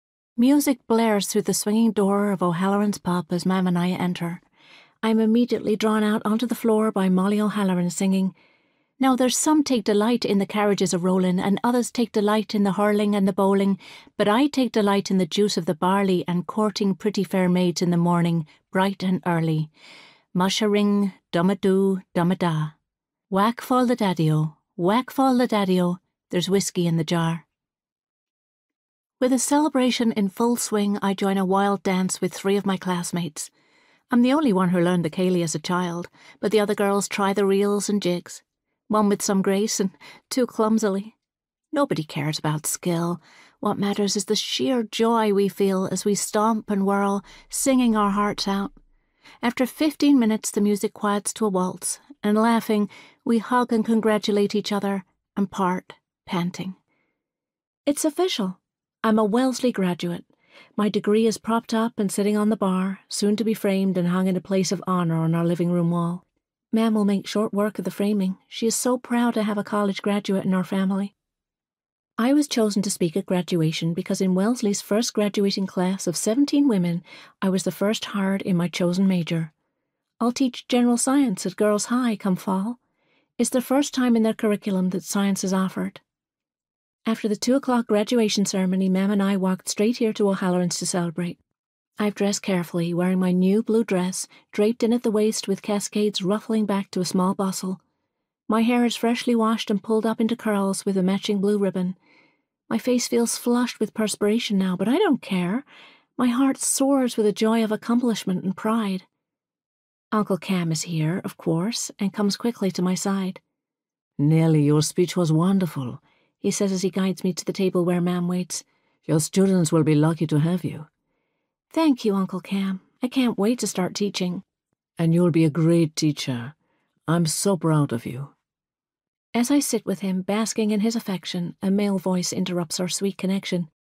Audiobook Sample: Silk Stocking
Three generations of Irish American women seek better lives in Boston, in this historical saga of hope, hardship, and love by the author of Lace Curtain.

Mary Boland suffered aboard a coffin ship to escape famine and make her way to America. Her daughter, Nellie, was born in South Boston—a beautiful girl, smart enough to graduate from college when few women, let alone women of her background, could. But her glowing future as a teacher is ruined when she becomes pregnant—and she is forced to cope on her own as the baby's father disappears after returning to Ireland to fight for his nation's liberation.

Her subsequent rushed marriage to an ambitious politician will throw Nellie's life far off course—and reverberate in her mother's and daughter's lives as well. Nothing will ever be the same in this gripping conclusion to the Daughters of Ireland saga—a tale of poverty and wealth, opportunity and danger, and family and forbidden love set against the tumultuous backdrop of the late nineteenth-century Gilded Age.